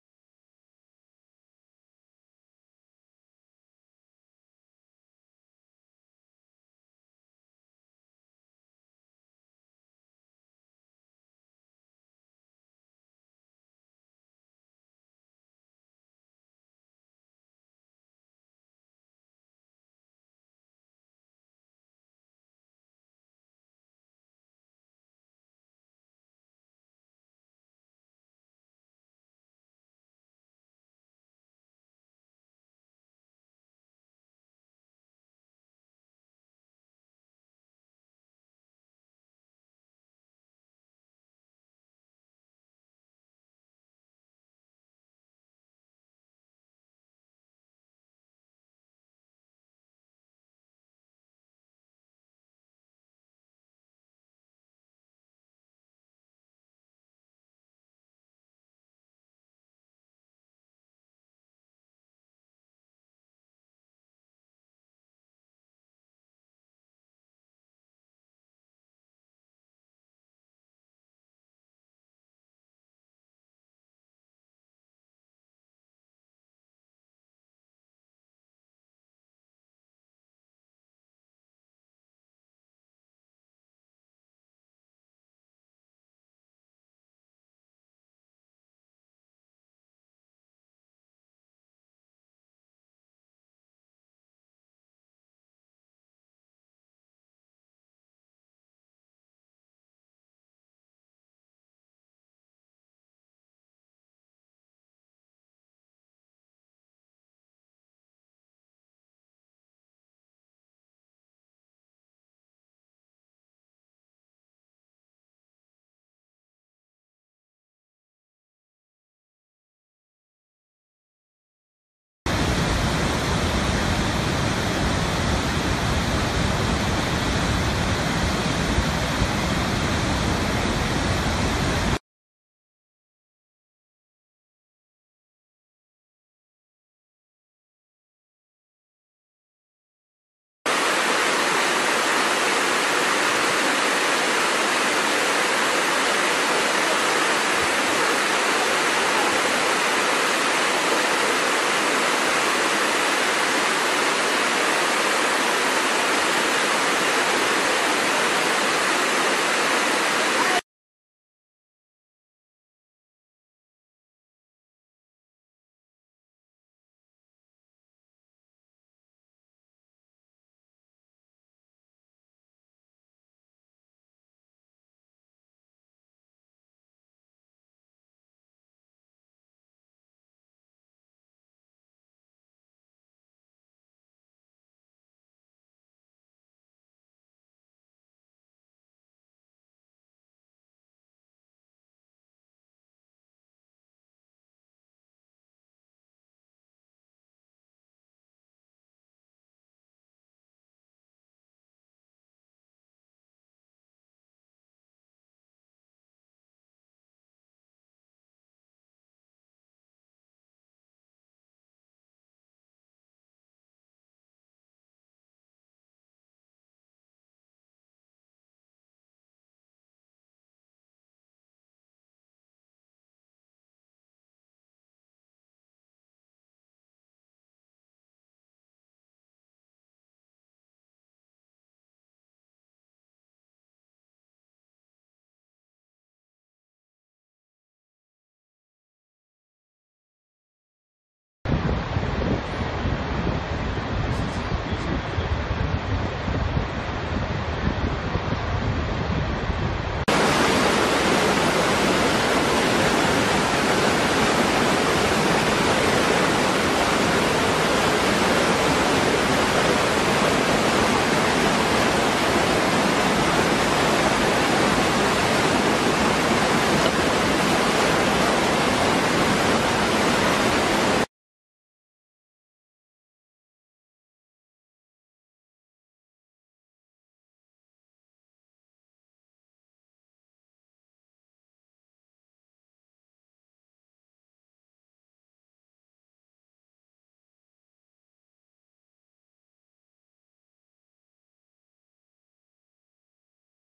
Amazing WaterFall Video - Free Stock Video
It's a free stock video of a waterfall and it's ready to use or re-upload in youtube